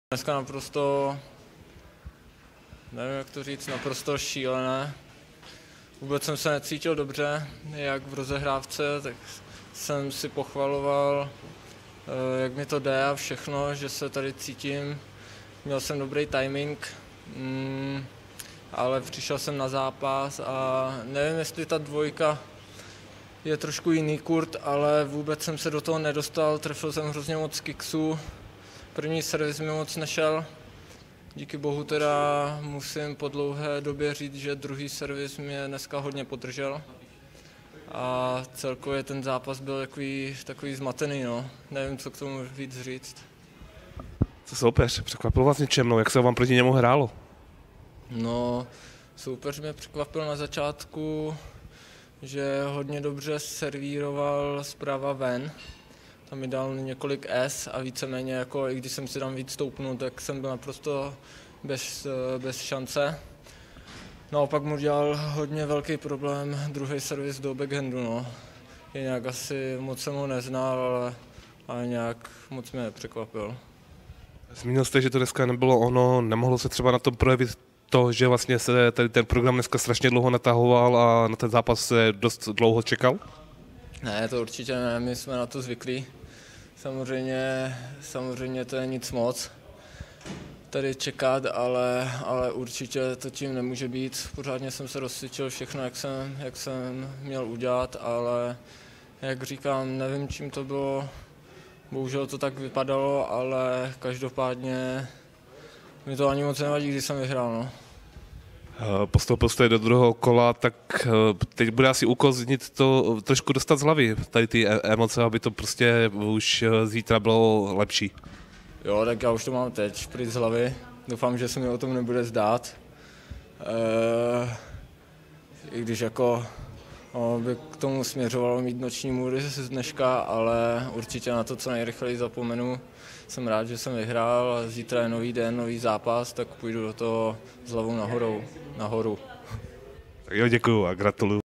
Petr Hájek po vítězství v 1. kole ITF Říčany
1. kolo ITF Říčany: PETR HÁJEK – Kiril Kivatcev (RUS) 6:3 6:4